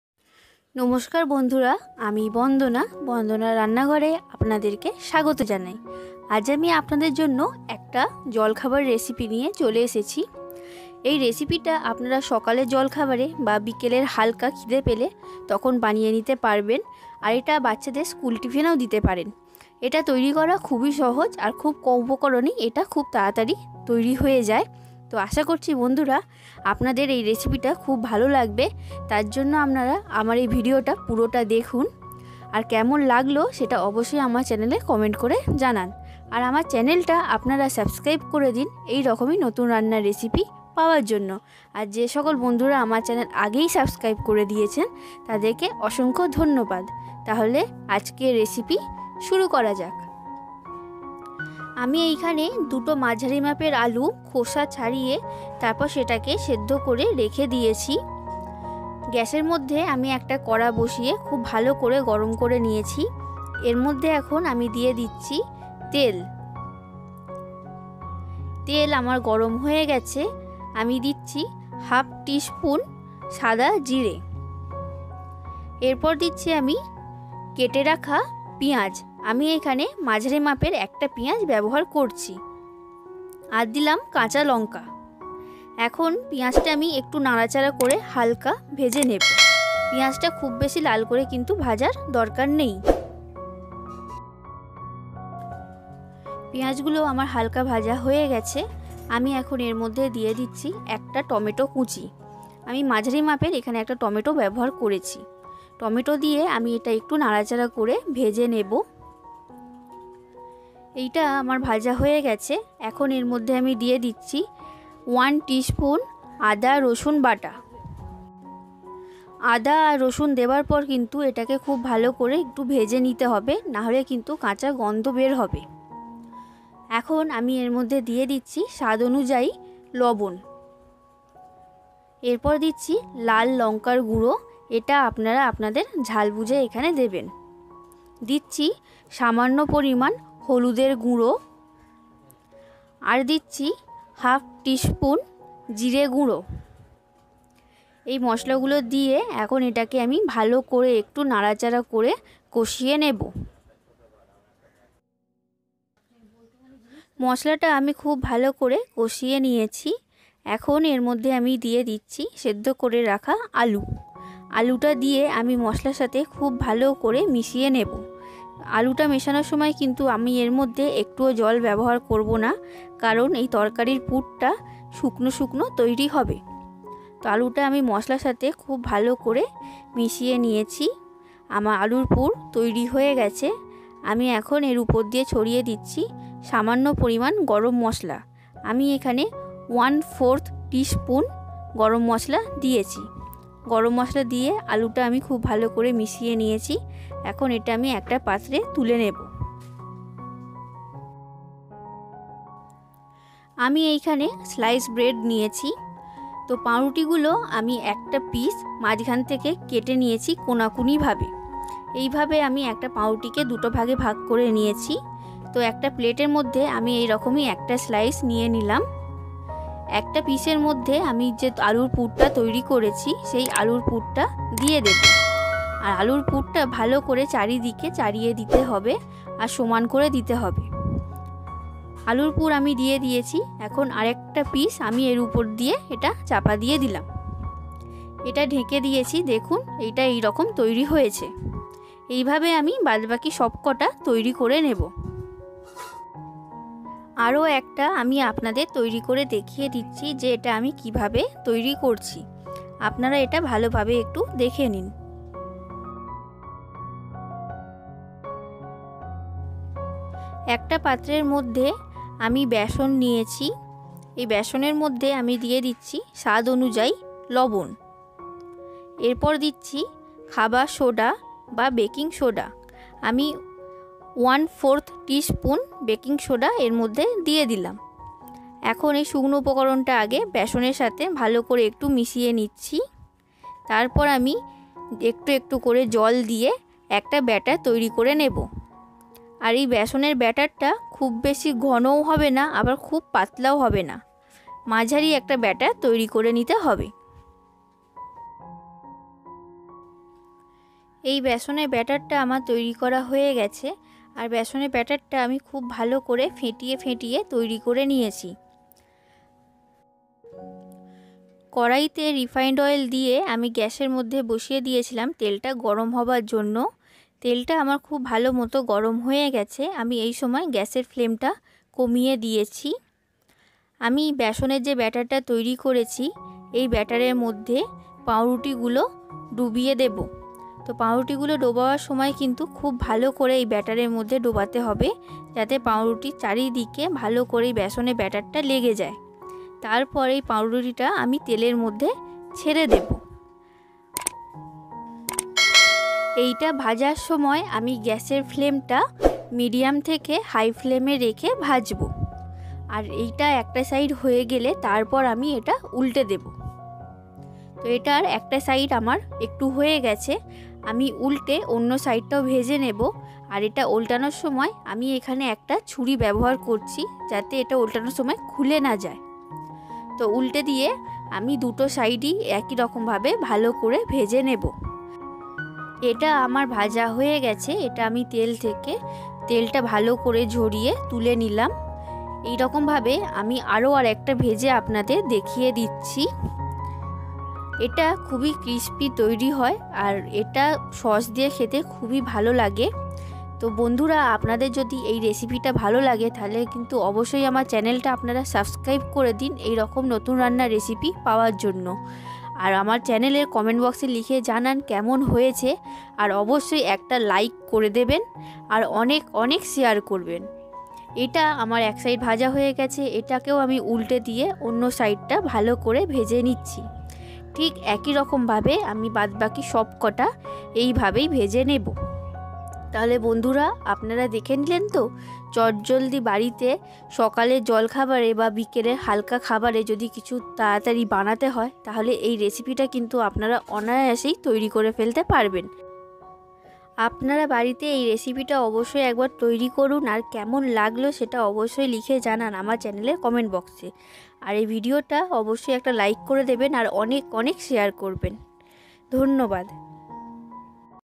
চটজলদি সকাল বিকালের নাস্তায় পুর ভরা পকোড়া থাকলে ছোটো বড় সবাই খুশি হয়ে খাবে॥Bread pakora recipe.
চটজলদি সকাল বিকালের নাস্তায় পুর ভরা পকোড়া থাকলে আর কোনো কথাই হবে না।এতটাই টেস্টি হয় এই খাবার টা।

আর কেমন লাগলো সেটা অবশ্যই লিখে জানান আমার চ্যানেলে কমেন্ট বক্সে।।

 ভালো লাগলে অবশ্যই এটা লাইক ও অনেক শেয়ার করবেন।।

আশা করছি আপনাদের সবার এই রেসিপি খুবই ভালো লাগবে।